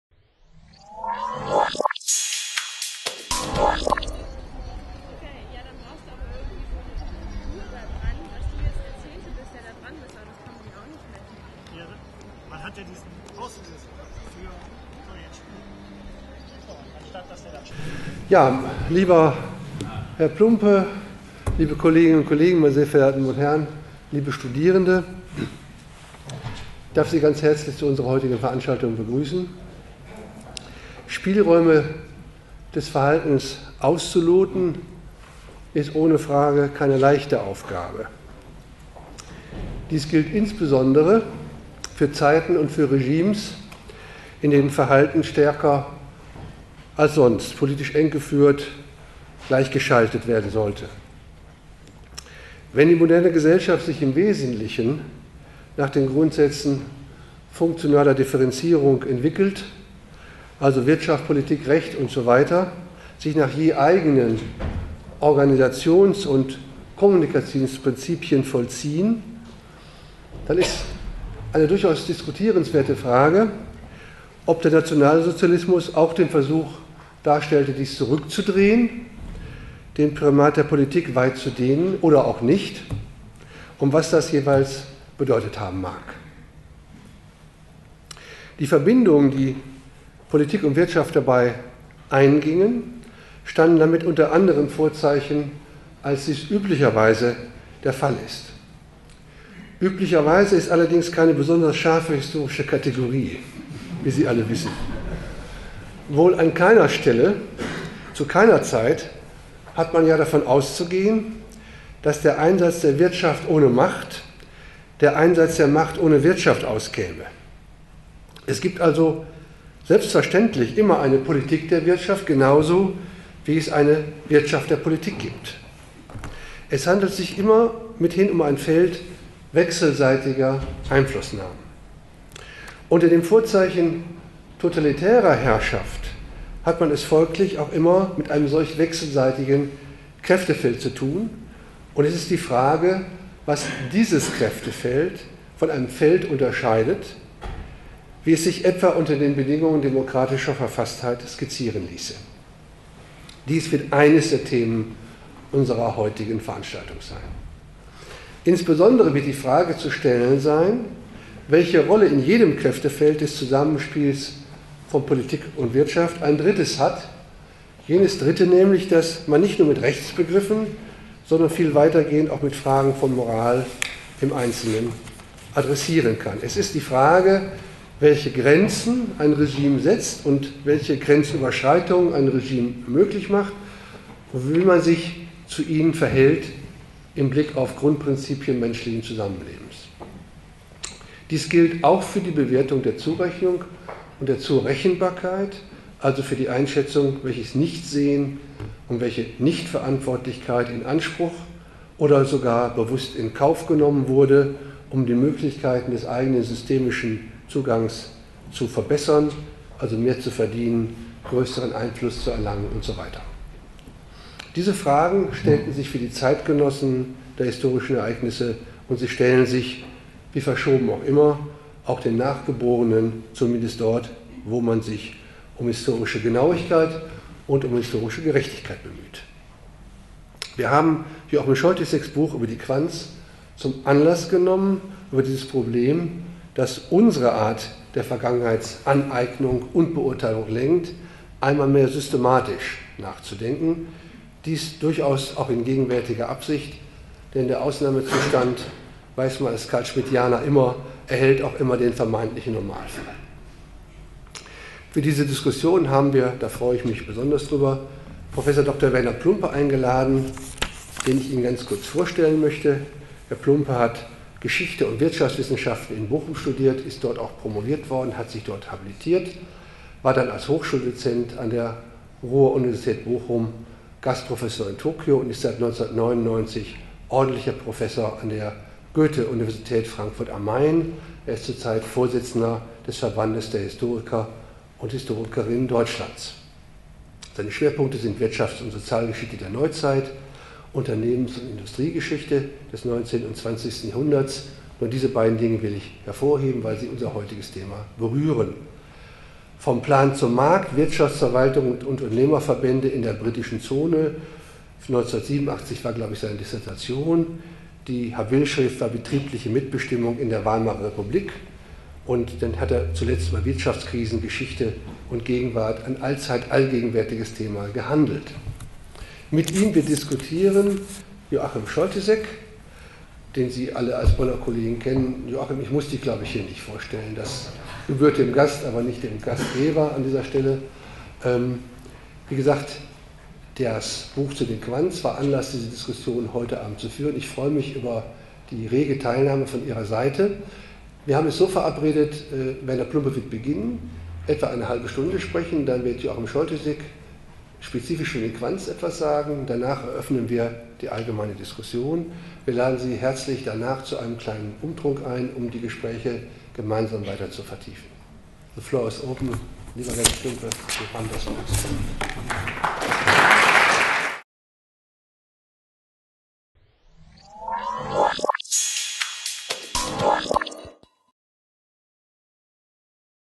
Quandts Spielräume - Einleitender Vortrag des Rektors.
Prof. Dr. Joachim Scholtyseck hat die Basis des riesigen Vermögens der Familie Quandt untersucht und eine enge Verbindung mit dem NS-Regime offen gelegt. Zu seinem Werk hat Scholtyseck auf Einladung der Uni Bonn vorgetragen.
Dieser Videopodcast zeigt die Einleitung des Rektors der Universität Bonn, Prof. Dr. Jürgen Fohrmann.
Hier finden Sie den Videopodcast des Vortrags von Prof. Dr. Werner Plumpe, Universität Frankfurt, aus der Veranstaltung mit Prof. Dr. Scholtyseck.

Hinweis:
Joachim Scholtyseck, Der Aufstieg der Quandts. Eine deutsche Unternehmerdynastie. Verlag C. H. Beck, München 2011, 1184 Seiten, ISBN-13: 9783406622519

Eine Produktion von Uni-Bonn.TV
Team: Denis Nasser